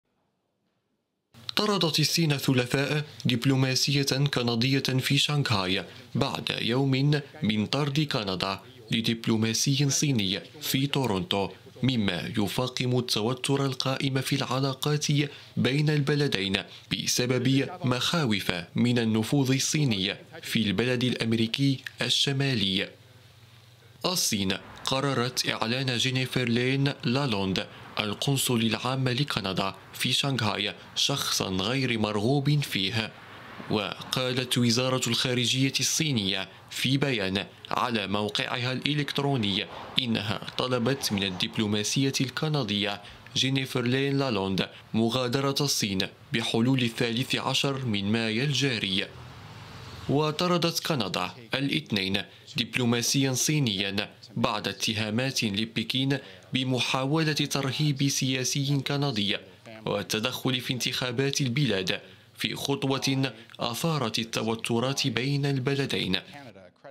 ردا على إجراء مماثل.. الصين تطرد دبلوماسية كندية من شنغهاي
لمتابعتنا

لتحميل التطبيق